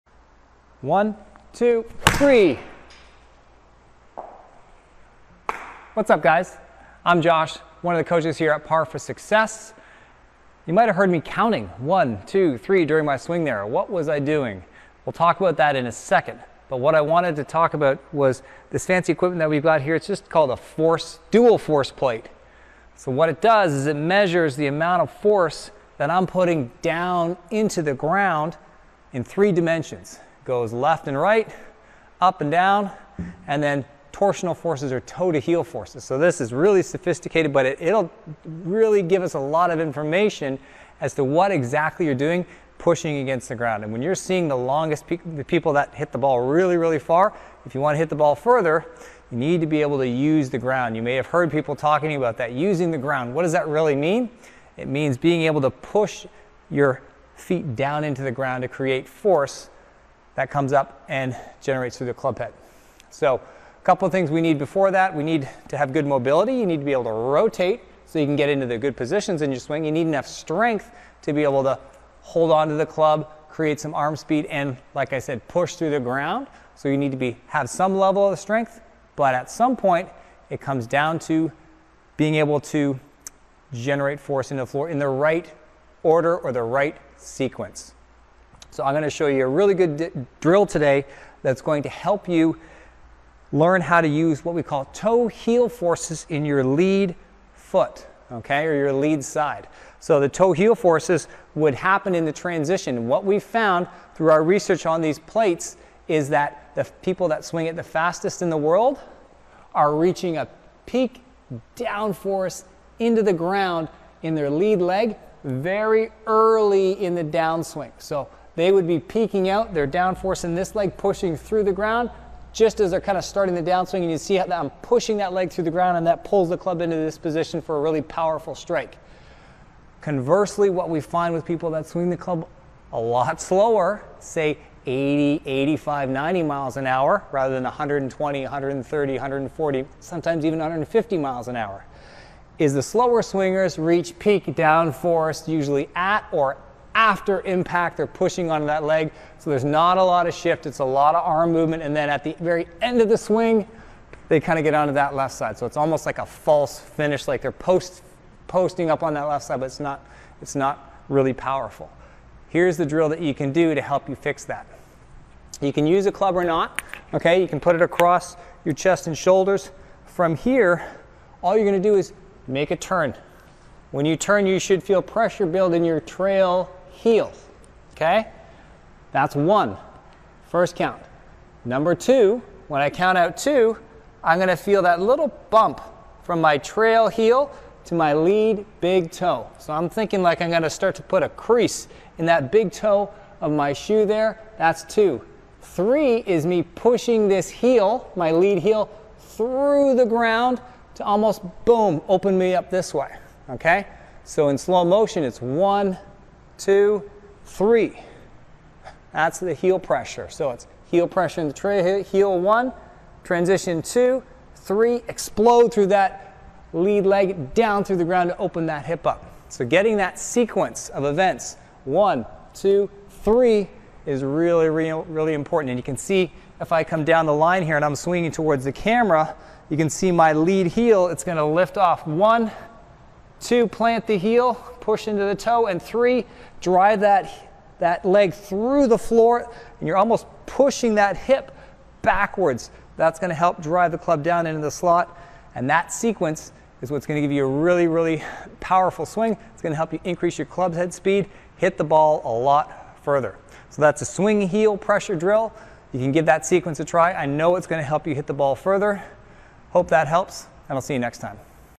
Quick Golf Drill to Increase Distance & Speed
If you would like to see how you compare to other golfers your age, check out our golf performance assessment here:

In this video, golf performance coach, Josh, goes over a quick drill that will have a huge impact on your golf swing and drastically increase your distance and speed. Through our research we have found this to be very important and it is usually the reason why pro's hit the fall so much faster and further.